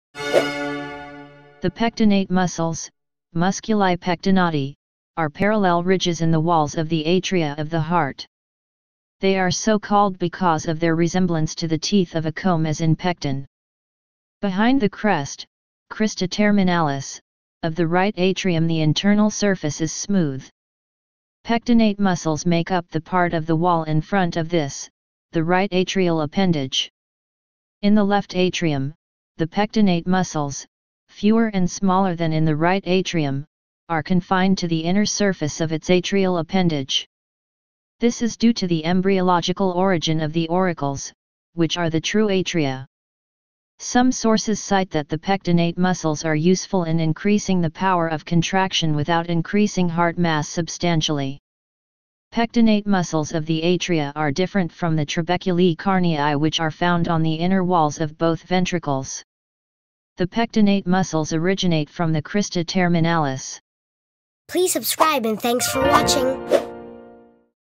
Pectinate muscles - Human Heart and Cardiology ❤️❤️❤️🔊✅
This video is about "Pectinate muscles". This video series is something special. We're fully delving into all things everything and all things about the HUMAN HEART and CARDIOLOGY!!!

The pectinate muscles (musculi pectinati) are parallel ridges in the walls of the atria of the heart. They are so-called because of their resemblance to the teeth of a comb as in pecten.